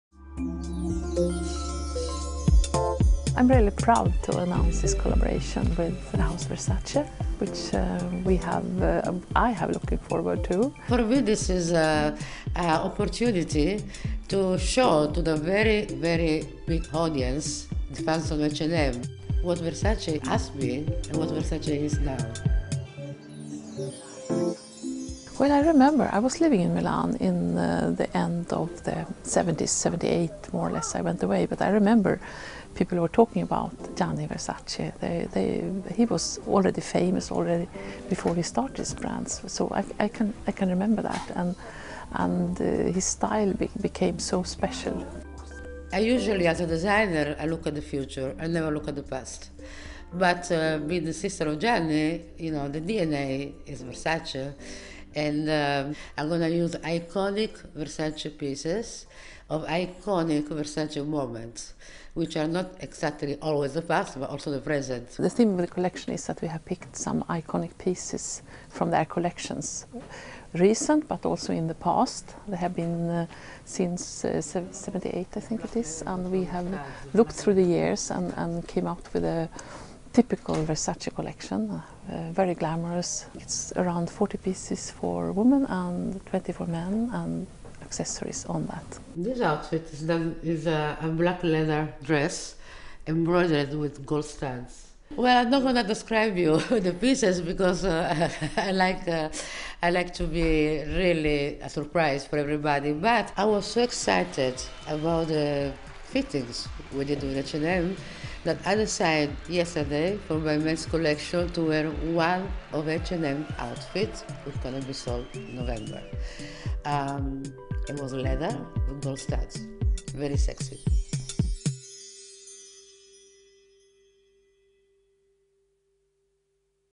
Versace für H&M
Creative Director Donatella Versace hat für H&M eine exklusive Damen-Kollektion und einige ausgewählte Modelle für Männer entworfen. Sie sollen ab 17. November in rund 300 H&M-Stores sowie online weltweit verkauft werden. Eine Pre-Spring-Kollektion soll ab 19. Janauar 2012 exklusiv in den Ländern vertrieben werden, in denen H&M einen Online-Shop betreibt.
In dem Video kommen Margareta van den Bosch, Creative Advisor bei H&M, und Donnatella Versace zu Wort. Weitere Infos rund um die Mode- und Textilbranche und